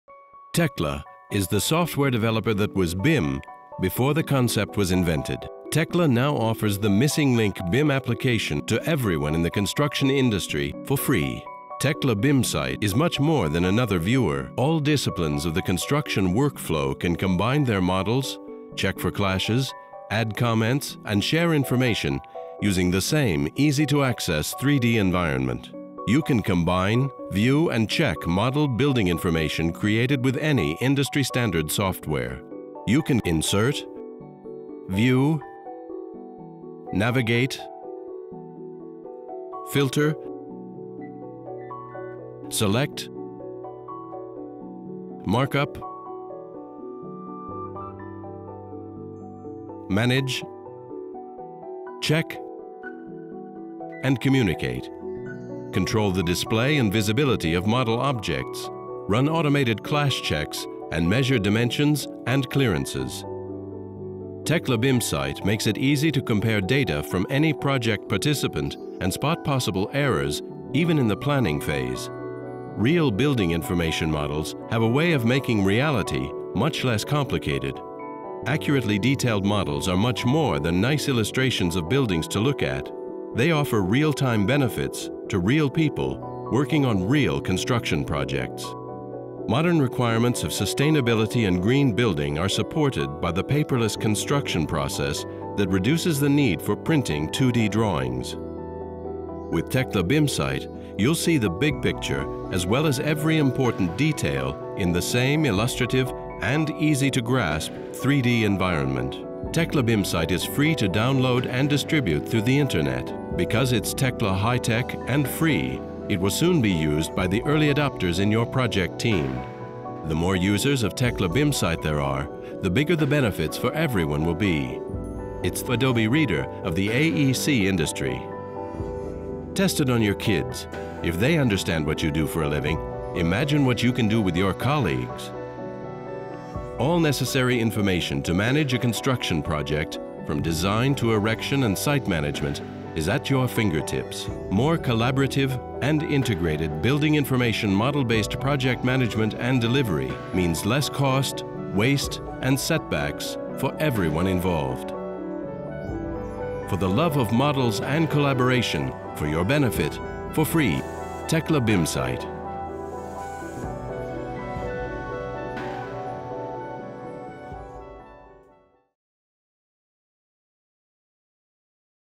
Tekla BIMsight - Free BIM software
Watch this 2.5-minute video clip to find out what Tekla BIMsight is all about. Tekla now offers the missing link BIM application for building information model-based project communication and management to everyone in the construction industry for free.